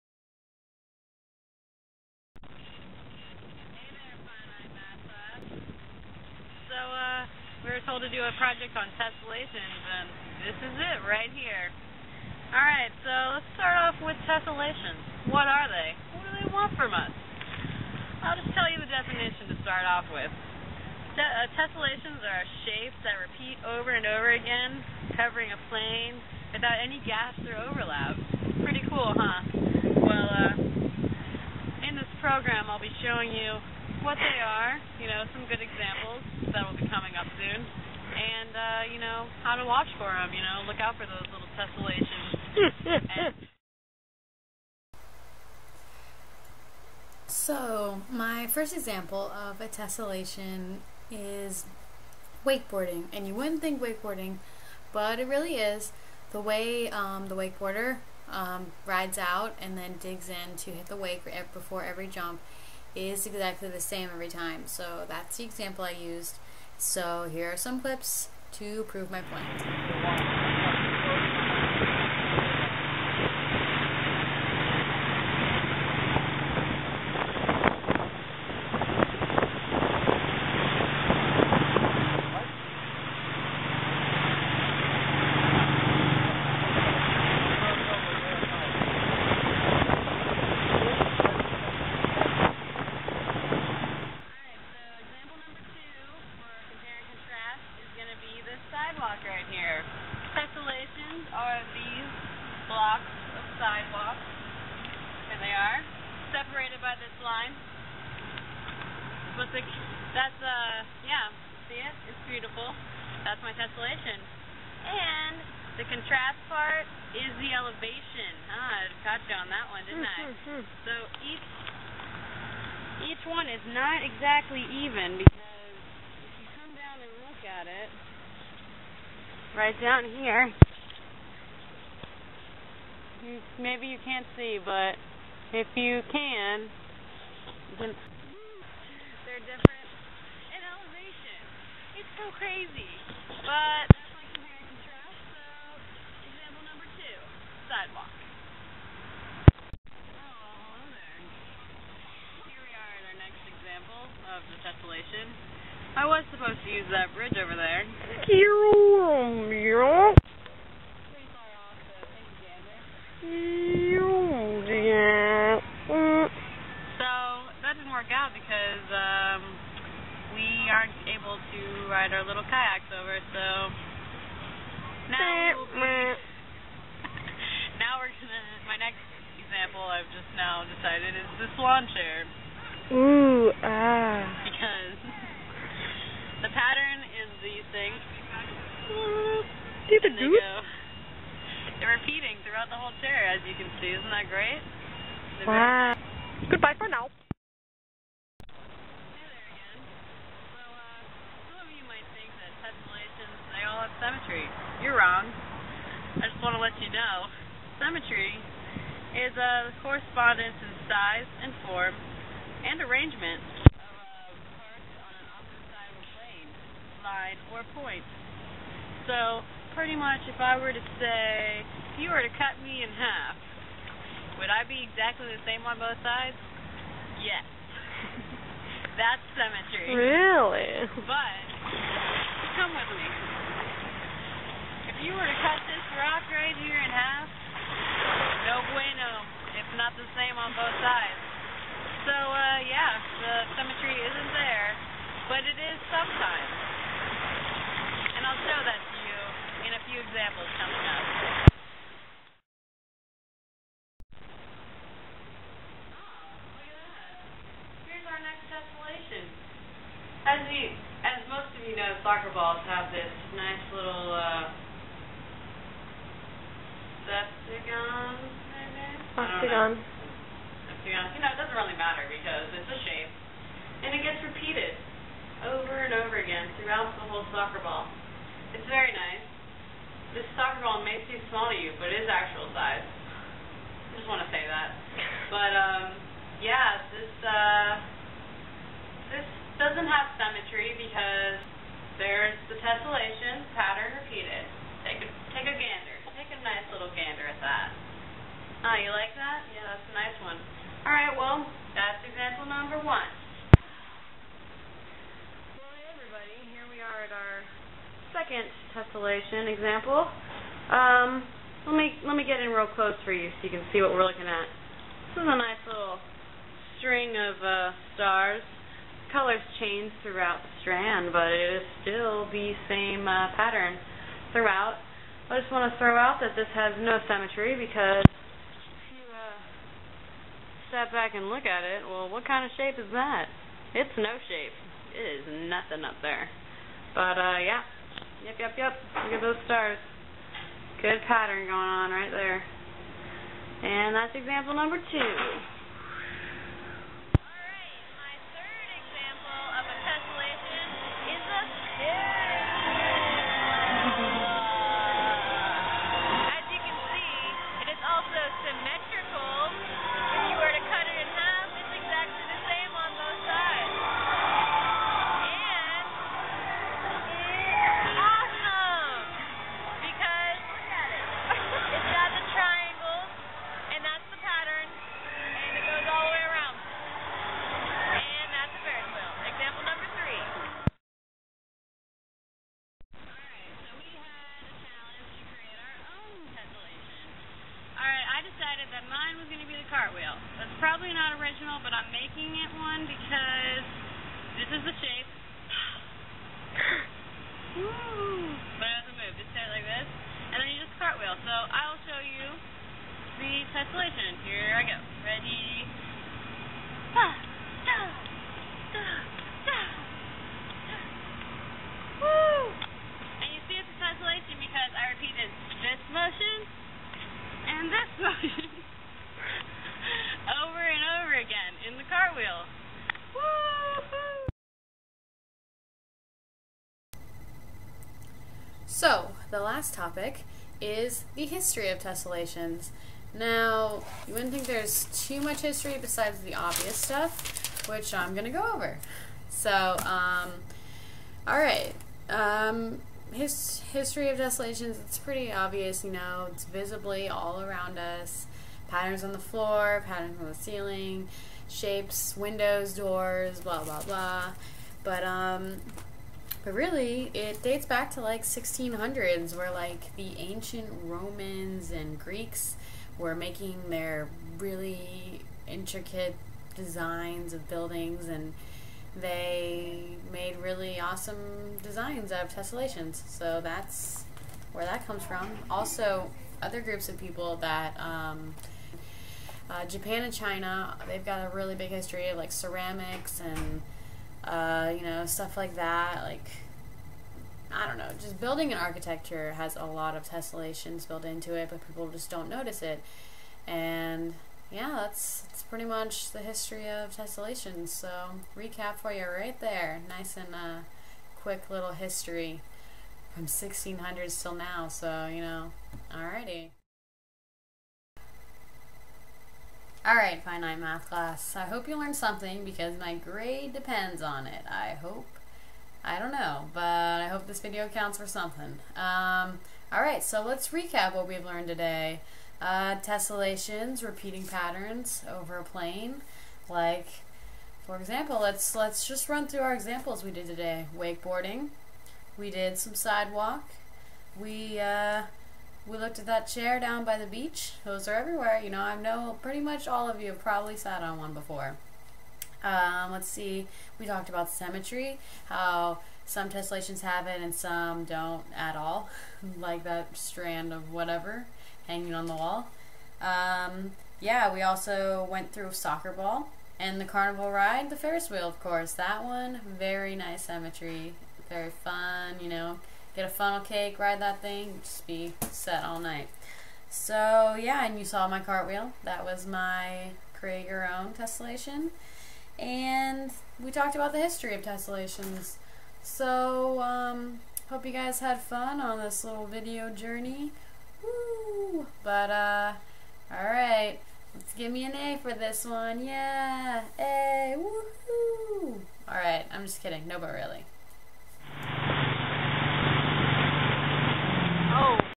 Tessellation Project: Finite Math
A project I had to do for Finite Math about tessellations.